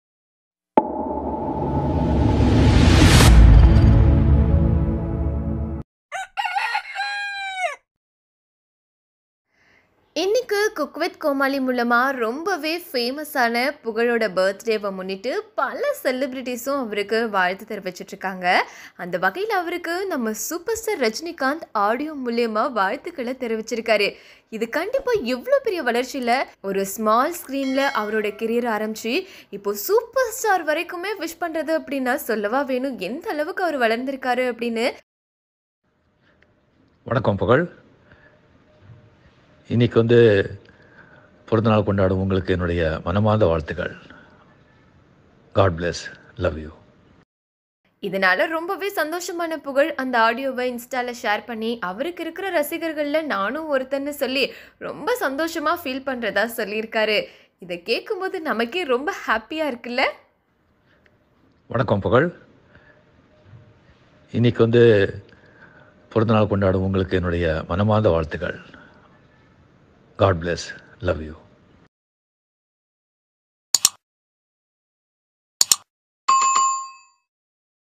Superstar Rajinikanth Wishing Pugazh | Pugazh Birthday wishes From Rajini | Audio
Audio of superstar Rajinikanth wishing Pugazh on his birthday and his feel for the wishes